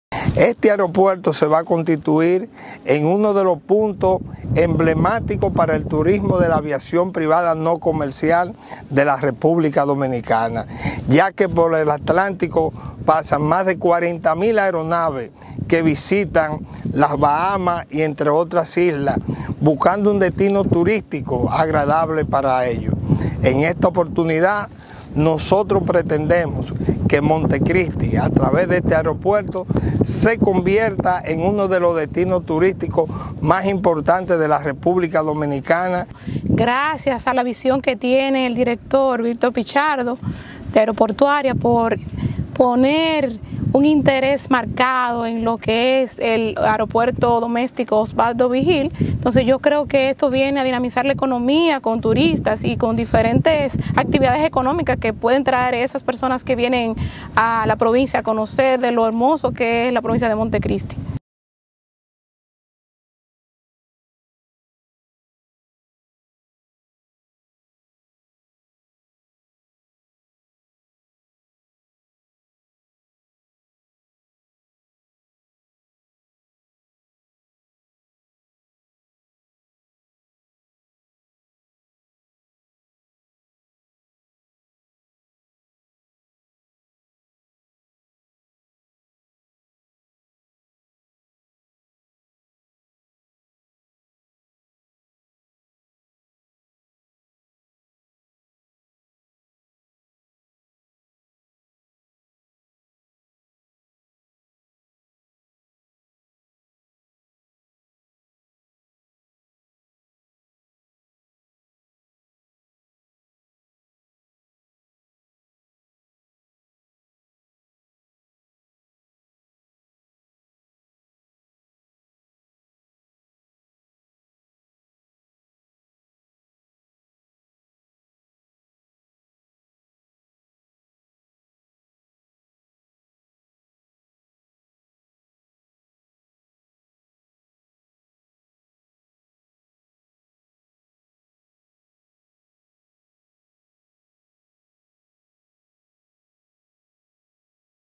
¿Para qué reparan el Aeropuerto Osvaldo Virgil, de Montecristi?
Víctor Pichardo, director del Departamento Aeroportuario, prometió convertir el Aeródromo Osvaldo Virgil, de Monte Cristi, se convertirá en un punto emblemático de la aviación privada no comercial, ya que por el Atlántico pasan más de 40 mil aeronaves que visitan las Bahamas y otras islas buscando un destino turístico agradable para ellos. Tras anunciar su reparación, Pichardo dijo que aspiran a convertir a Montecristi en uno de los destinos turísticos más importantes de la República Dominicana.